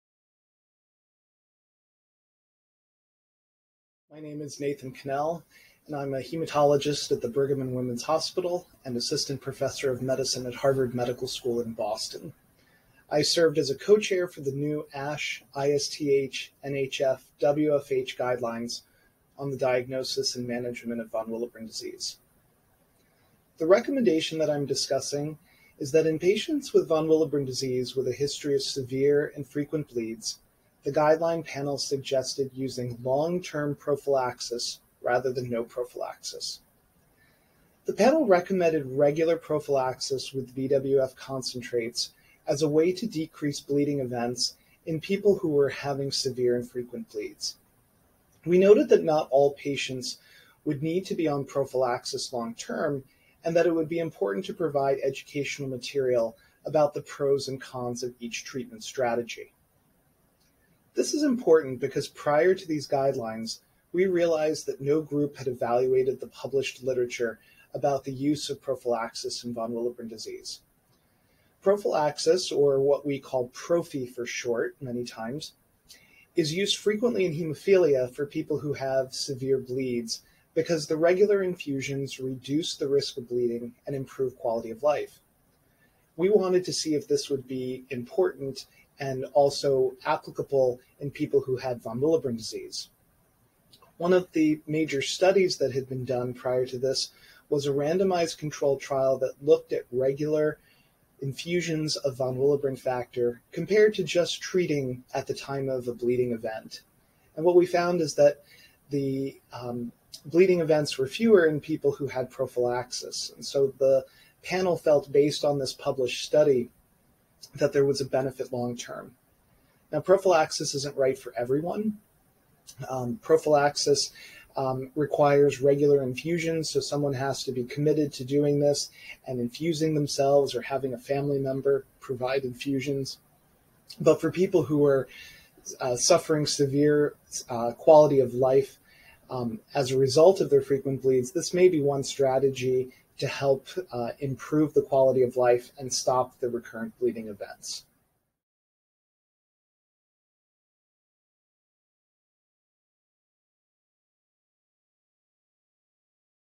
Nathan Connell - VWD Guidelines
In patients with VWD with a history of severe and frequent bleeds, the guideline panel suggests using long-term prophylaxis rather than no prophylaxis.